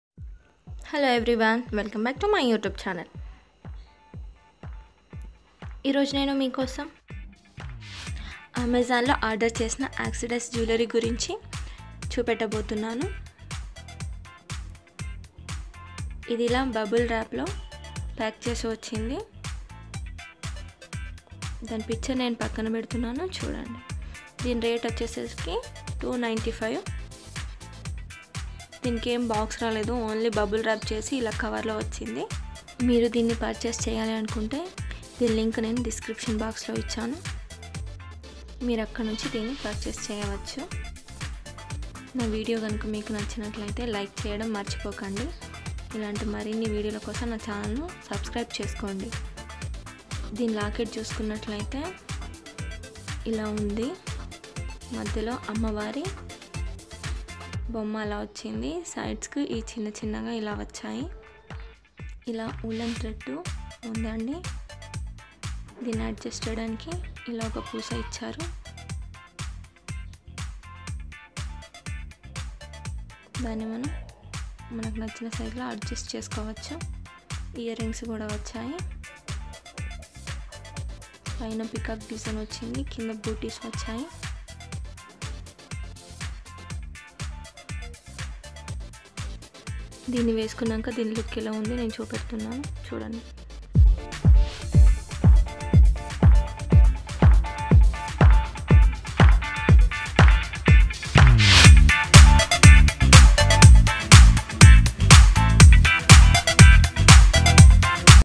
Amazon oxidised jewellery haul // genuine review by fashion life with meena
Hello everyone

 Wellcome to the fashion life with meena you tube channel

           Today's video is all about trendy jewellery from amazon. This is oxidised German silver necklace and earrings elegant set from brand kaiser. I purchased this for 295/- only

  Please check the below link
Kaizer Jewelry Antique German Silver Tribal Cotton Thread Jewelry Necklace Earring Set for Women & Girls.(Valentine Gift Special). (Black)

Hope you will enjoy this amazon jewellery haul video

My videos :-